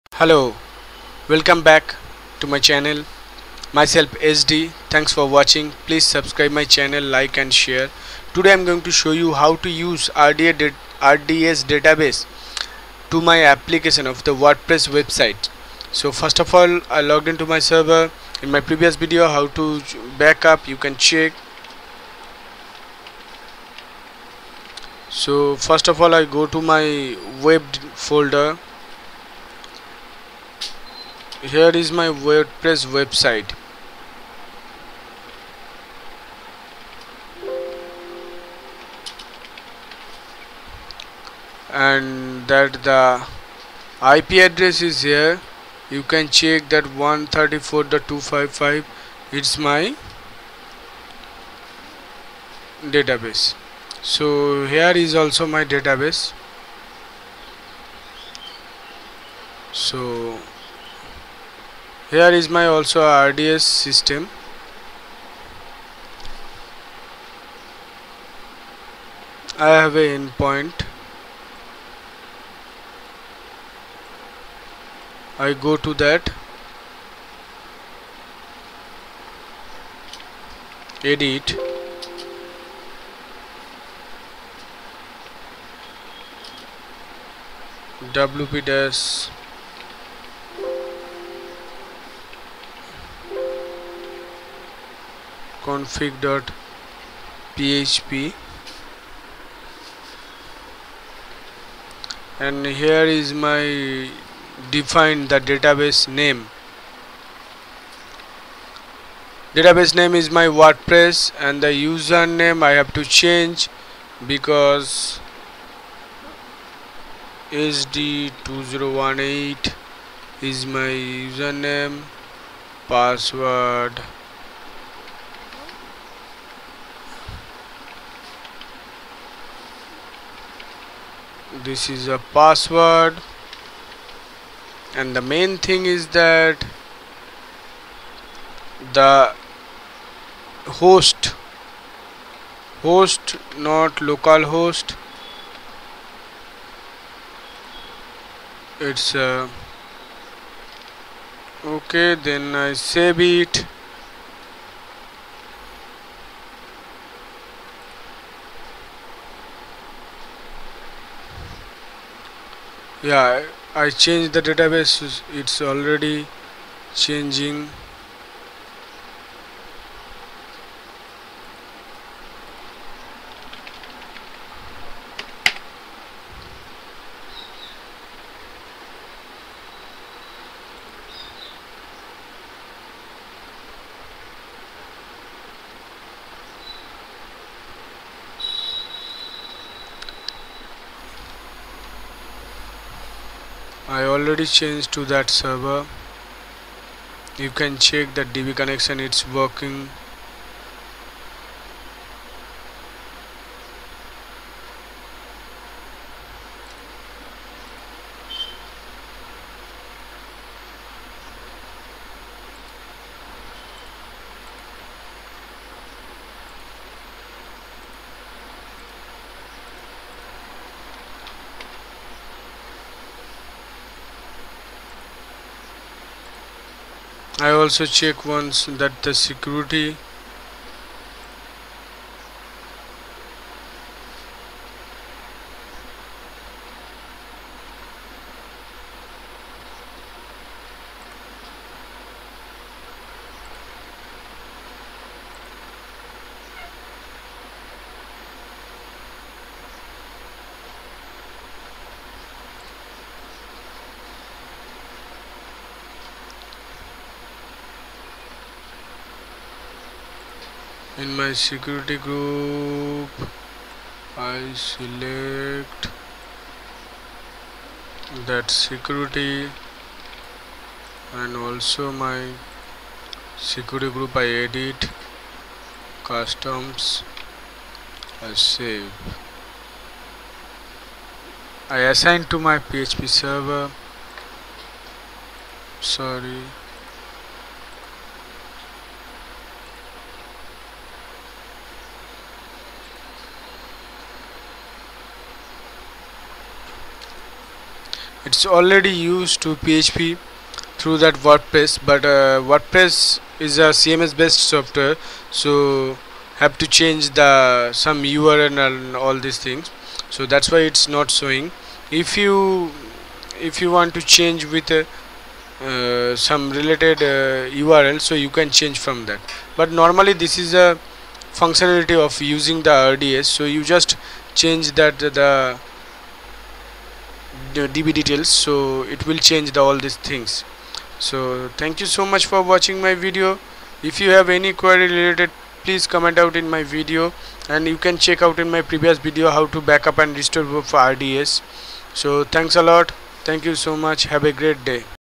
How to deploy a Wordpress website with amazon RDS Database
Using Amazon RDS with your WordPress installation
WordPress on AWS EC2 with RDS Database
How to Launch 'Wordpress Multi-tier with Amazon RDS' on AWS